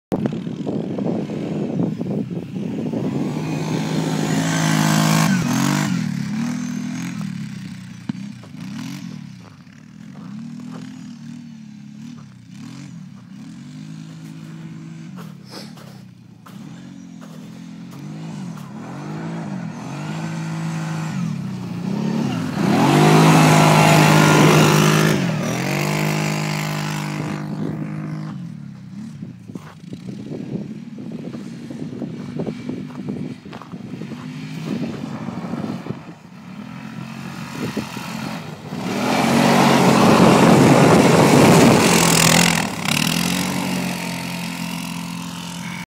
Honda Rincon 650 på hal is
Vintern 2011
Honda Rincon & Kawa 700 på Lilla Bjursjön

Winter 2011, nice day on the ice.